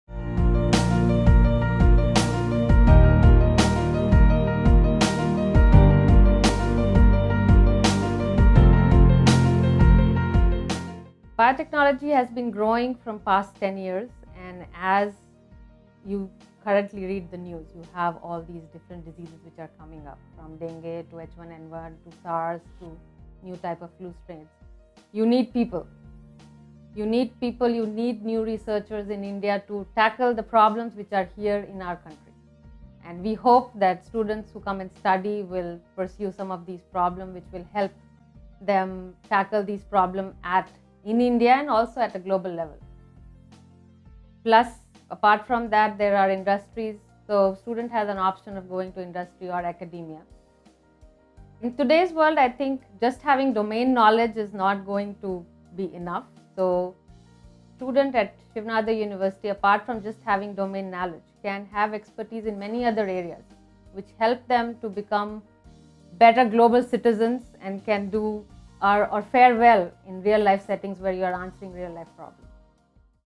Shaping careers in Biotechnology
Richa Priyadarshini, Assistant Professor, School of Natural Sciences, on career prospects in the biotechnology sector and how Shiv Nadar University prepares its students.